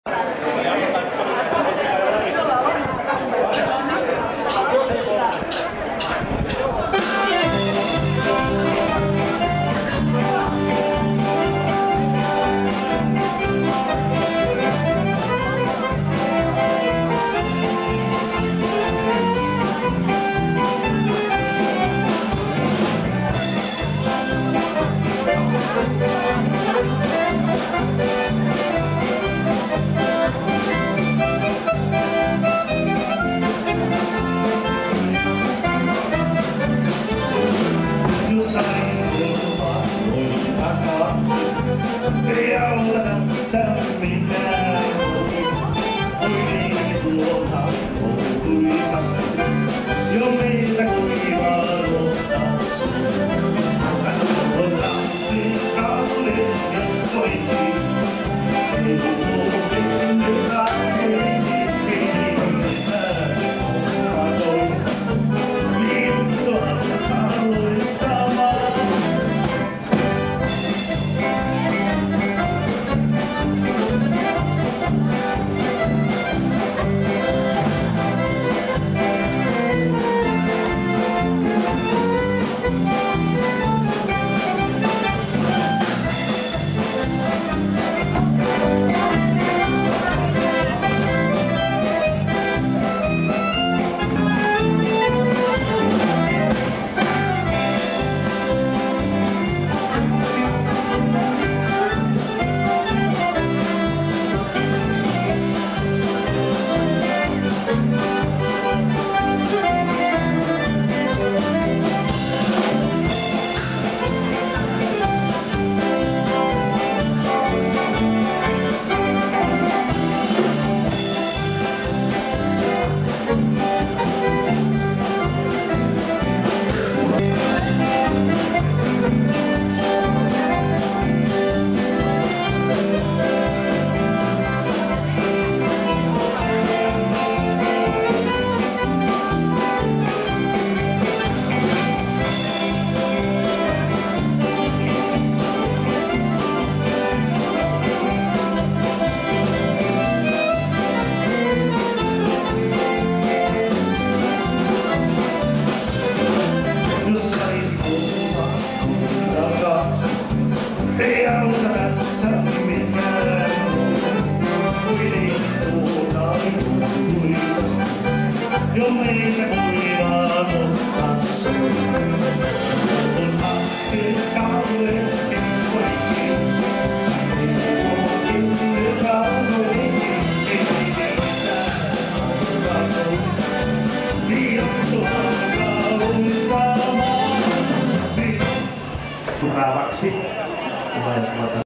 Humpa på Skeppsbron i Lovisa, Finland 24.6.2011
Midsommar afton på Skeppsbron i Lovisa, Finland. Orkestern spelar humpa.